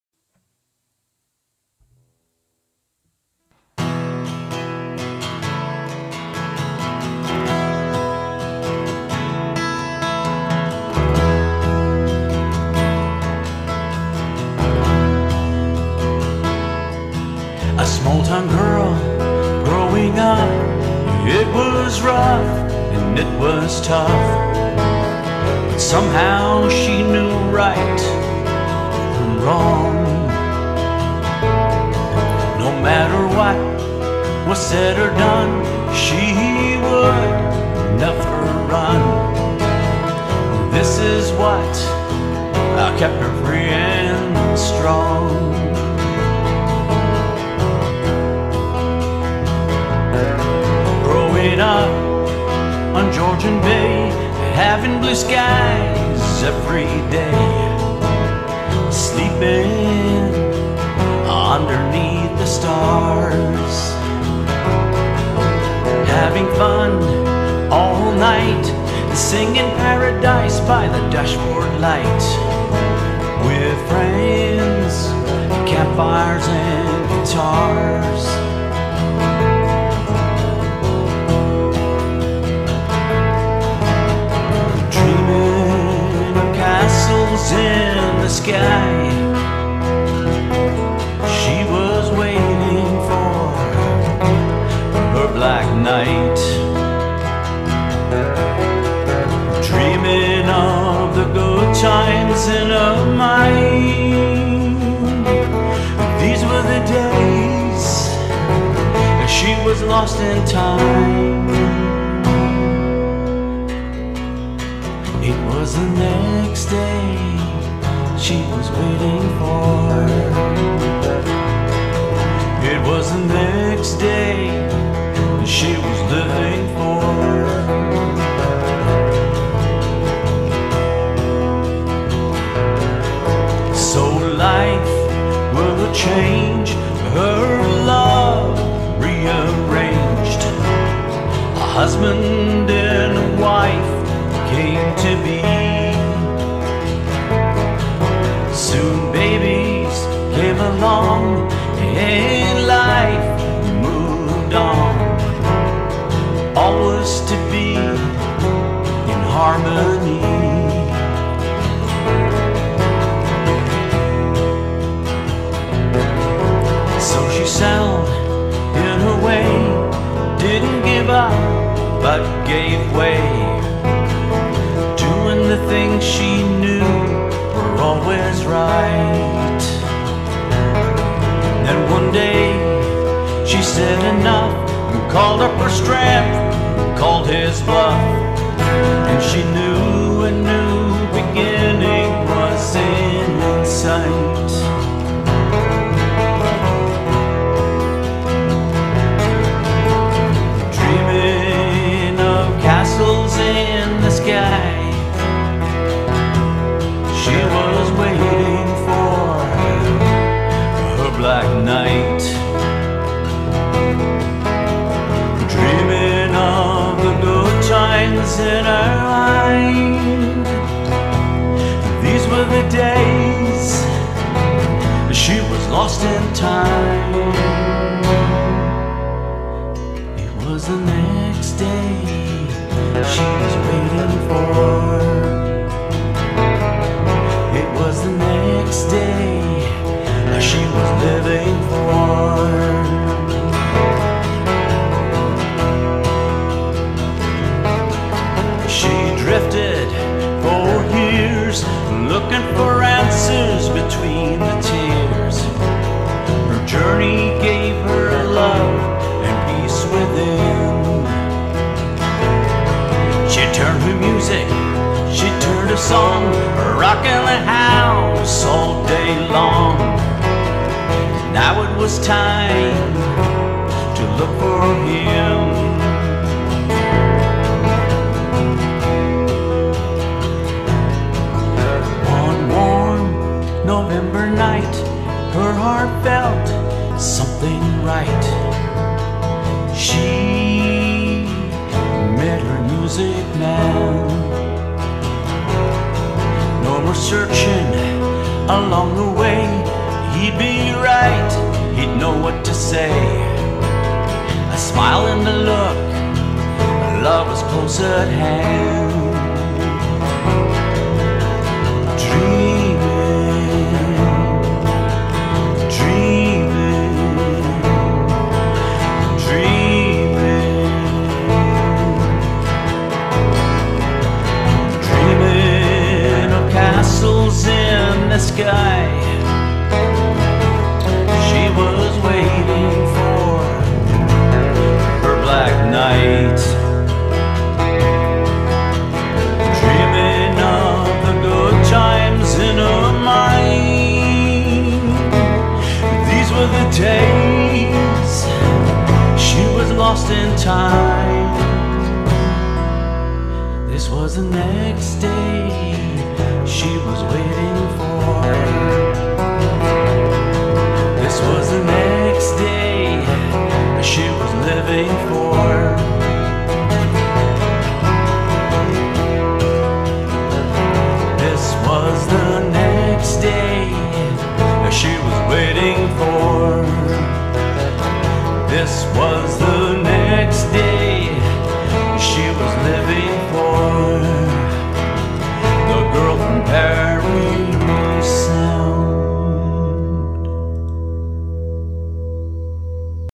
The Girl From Parry Sound
The journey of a young girl growing up on Georgian bay... the life she wanted was different then the life she was leading...staying true to herself in spite of the hardships...becoming the woman she wanted to be...free and strong...